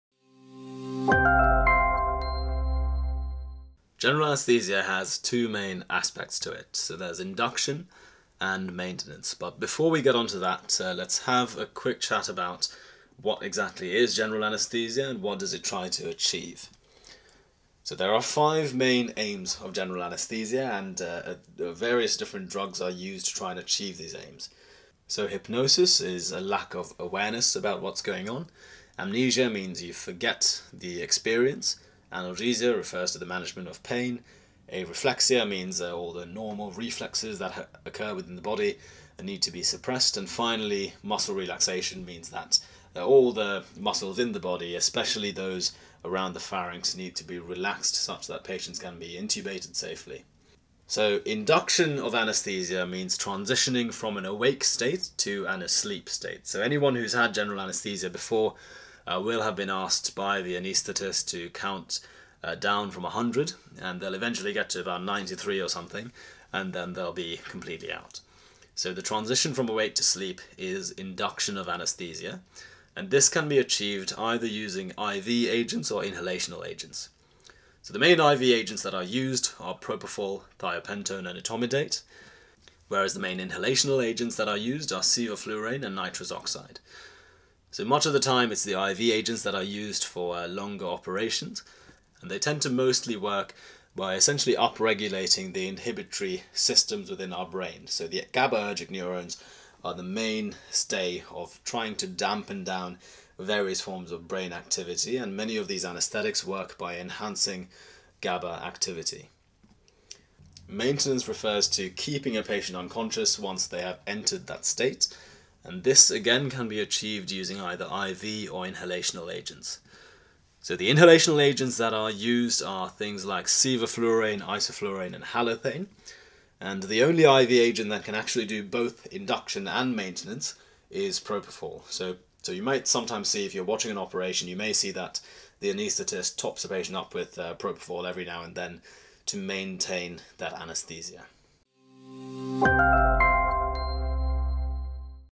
Principles of General Anaesthesia
In this short tutorial, we will be going through the basic principles of general anaesthesia.

Like this content?